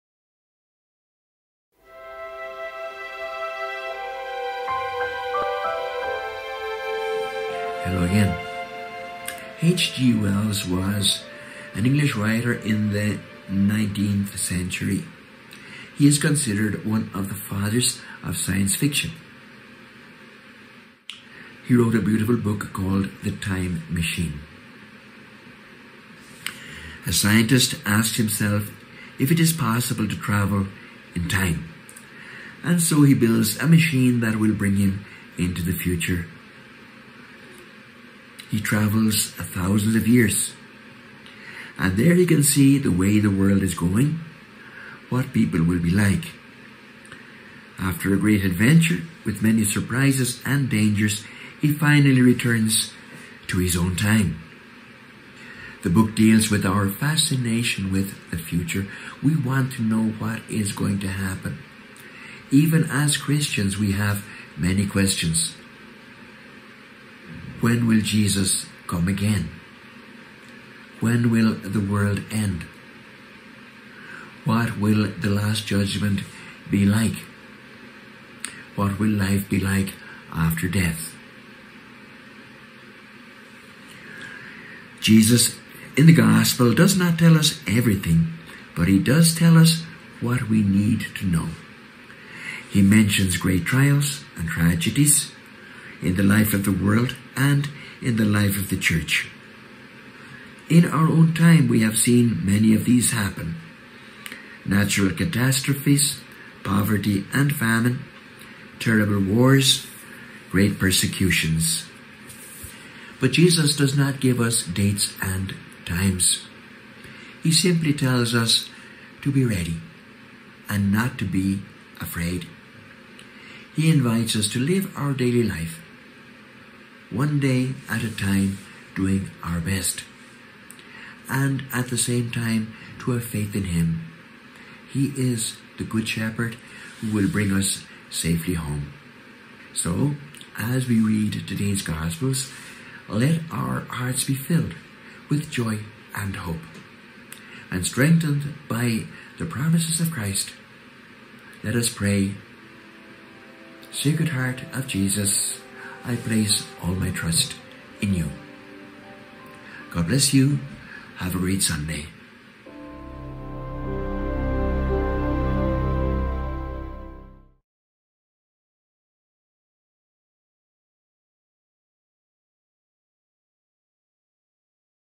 The Time Machine (Lk 21: 5-19)
Sometimes we might wonder what the future holds in store for us. This is today's topic with Fr. Vincent.